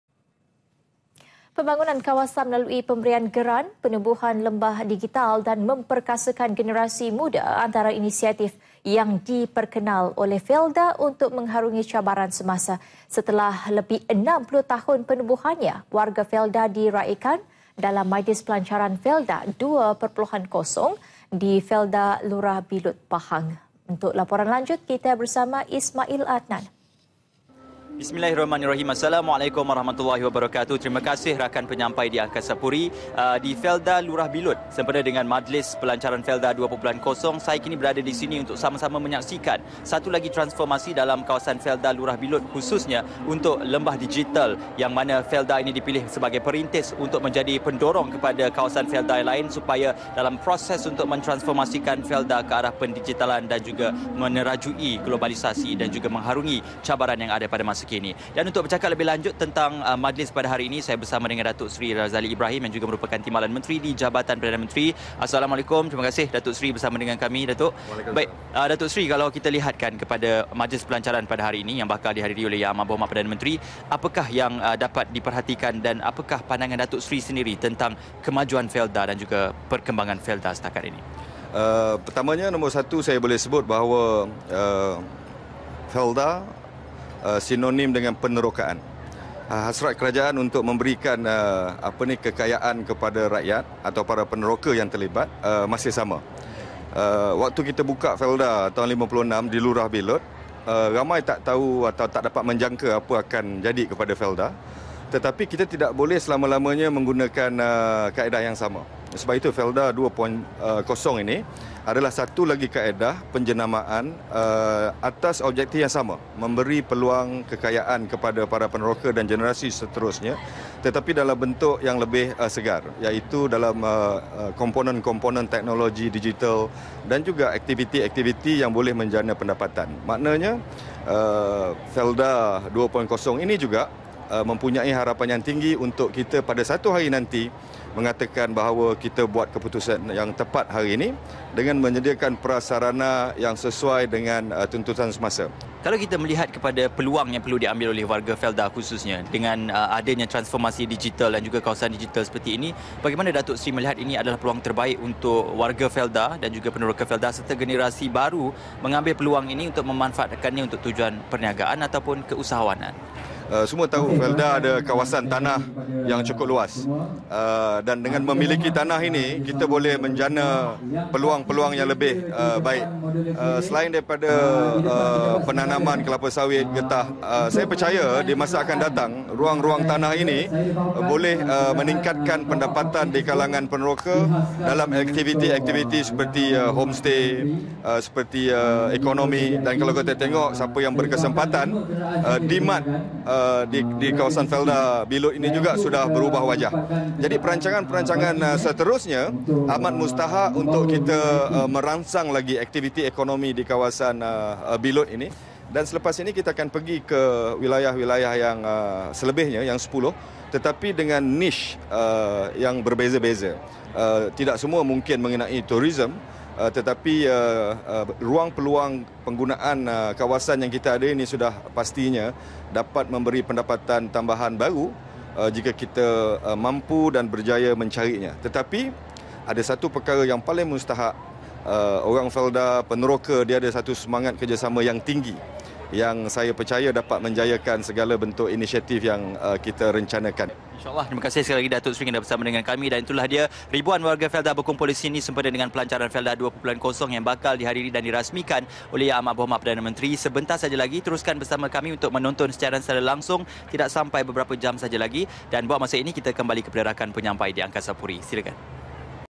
LANGSUNG DARI PAHANG SEMPENA MAJLIS PELANCARAN FELDA 2.0 DI FELDA LURAH BILUT 1PTG [4 FEB 2018]
Pembangunan kawasan melalui pemberian geran, penubuhan lembah digital dan memperkasakan generasi muda antara inisatif yang diperkenal oleh FELDA untuk mengharungi cabaran semasa.

Setelah lebih 60 tahun penubuhannya, warga FELDA diraikan dalam majlis Pelancaran Felda 2.0 di Felda Lurah Bilut, Pahang.

Untuk laporan lanjut kita bersama Ismail Adnan .